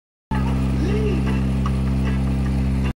Captain Pierce Is Cruising
It is a nice day for a cruise. Check the sails. Check the wind. Enjoy the weather it is cruising time. Don't forget to put a bandana under your hat so you don't get sunburnt.